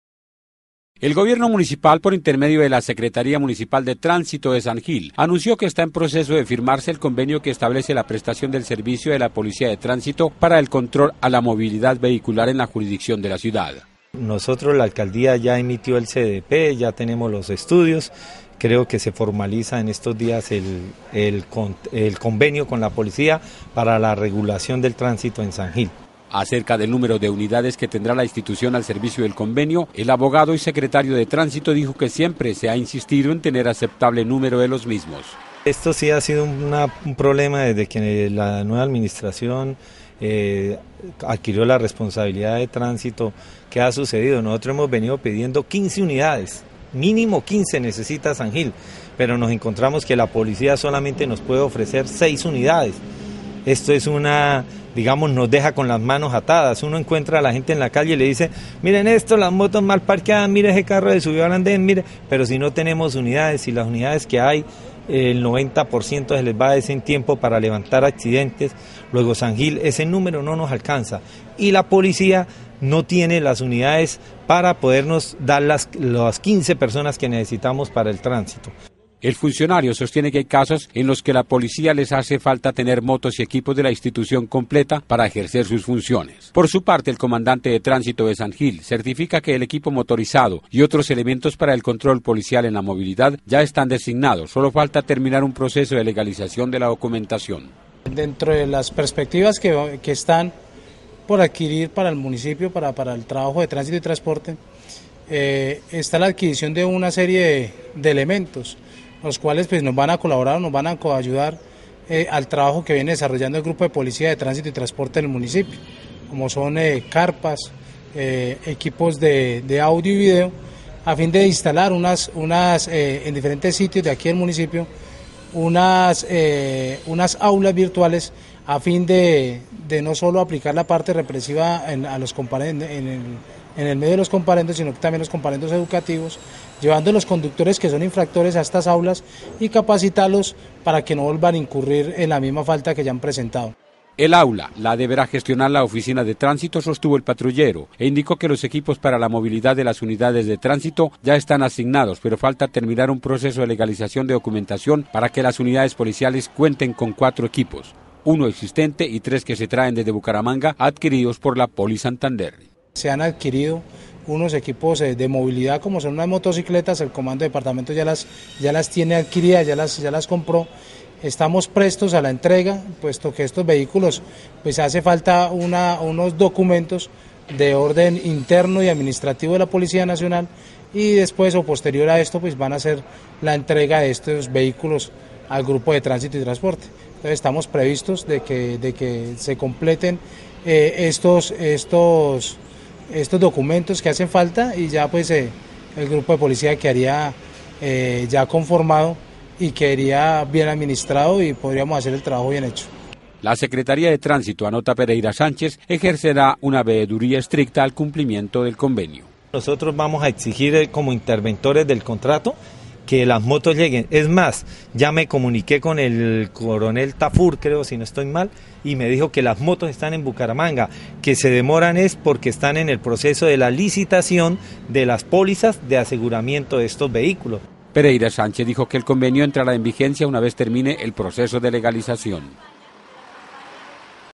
Firmarán convenio Administración Municipal y Policía de Tránsito en San Gil.
La secretaría de tránsito de San Gil confirma que está próximo a firmarse el convenio alcaldía y la policía de tránsito que controlará la movilidad vehicular en la ciudad, resta la legalización del documento para recibir elementos básicos para el control y la pedagogía.